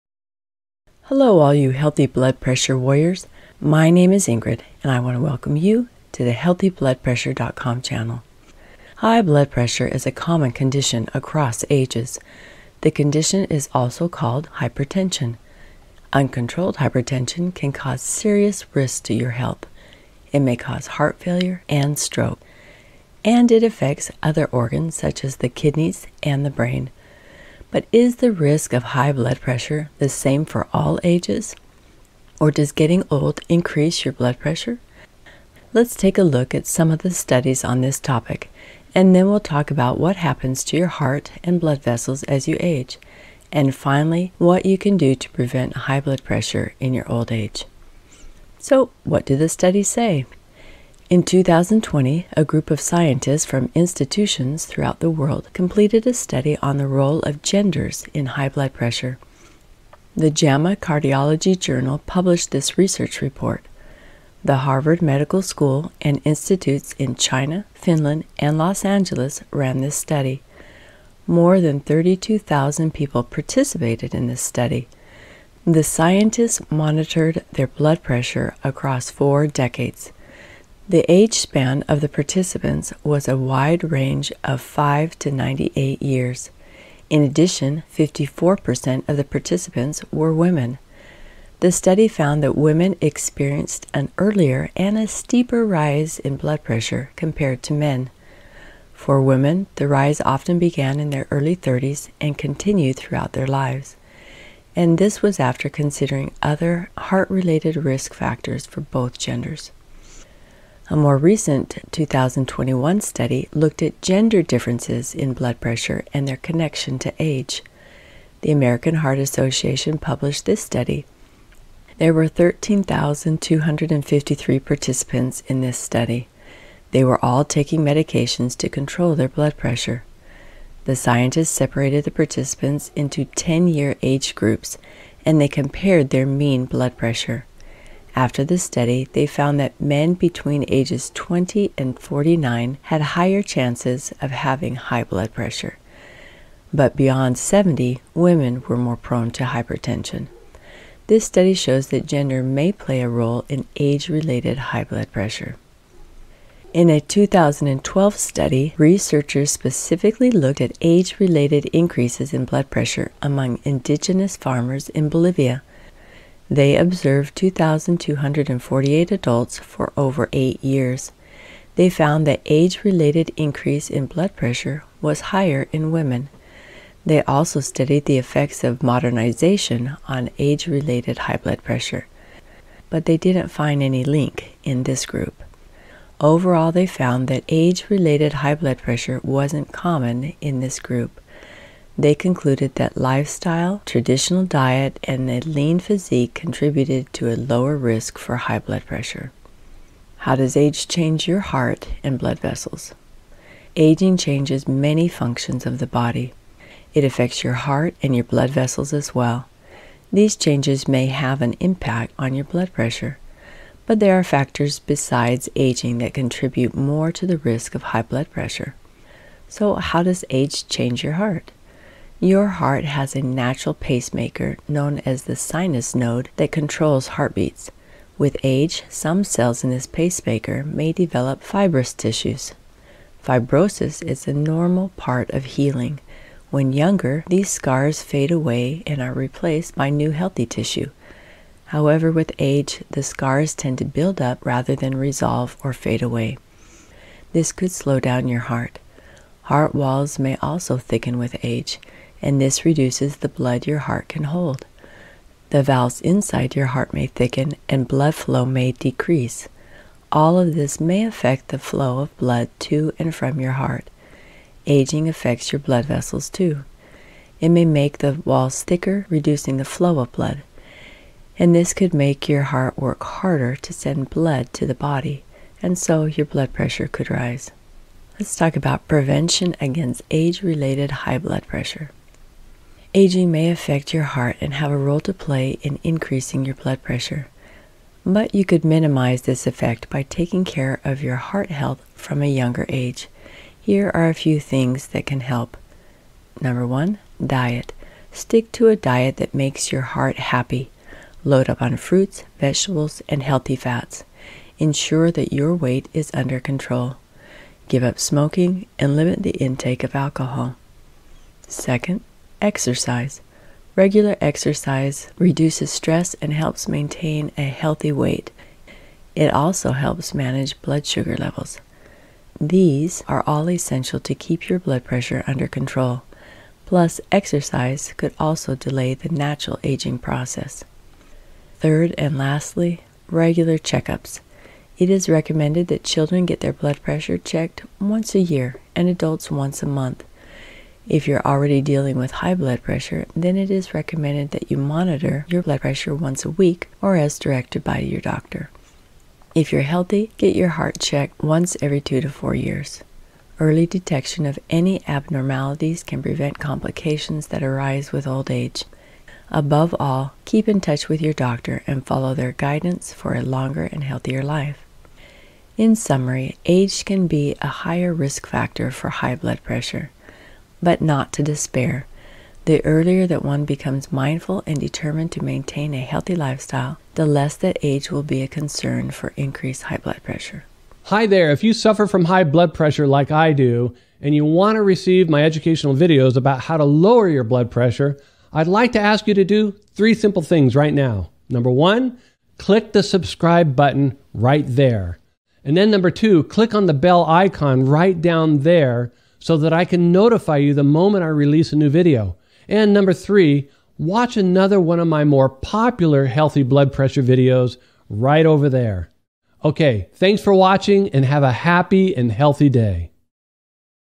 Does Getting Old Increase Blood Pressure?
Although you might think you know the answer already, the studies findings might surprise you.  In this video, you'll learn what age has to do with blood pressure and how you can hold back time on your body and look forward to better health. If you suffer from high blood pressure, this is a video to watch.

SOURCES:
Is High Blood Pressure in Older Age Inevitable?

Does Blood Pressure Inevitably Rise With Age?

How does blood pressure change with age?

Uncontrolled high blood pressure shifts with age, hitting men and women differently

HOW DOES THE CARDIOVASCULAR SYSTEM CHANGE WITH AGE?